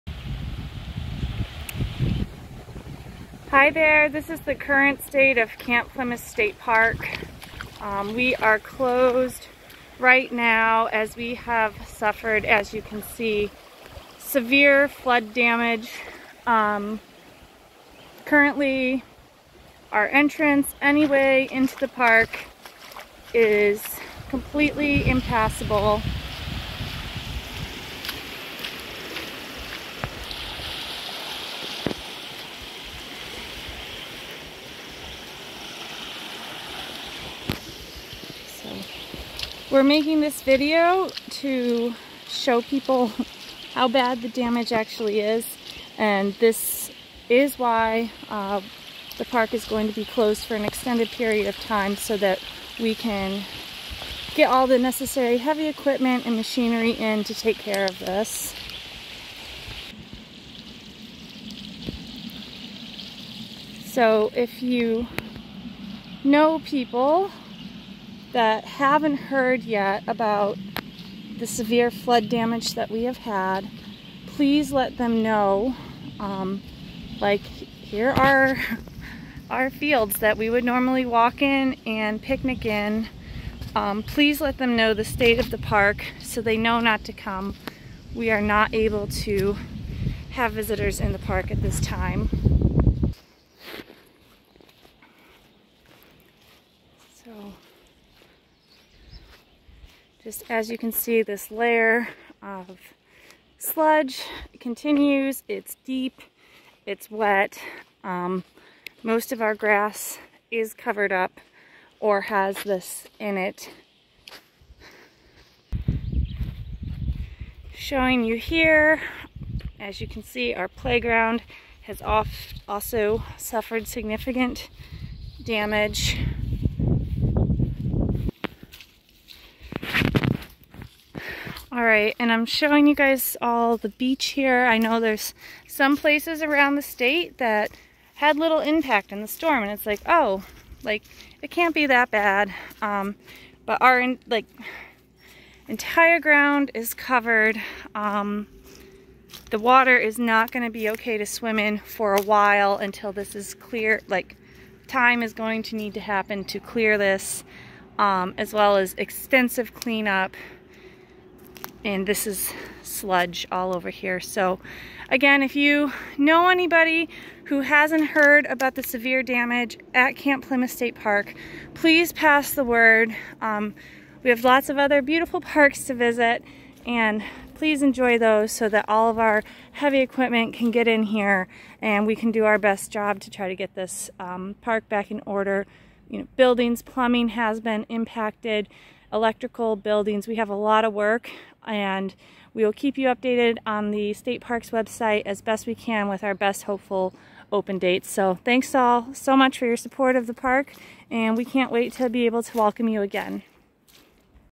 Camp Plymouth Flood Closure July 2023
Camp Plymouth State Park suffered severe flooding as of July 2023.

The park will be closed for an extended period of time - at least until mid-August.

At this time the park cannot accept visitors. Please pass the word to friends or people that might not realize the extent of the damage. We will need to get a lot of equipment into the park and please give them the space to make the repairs.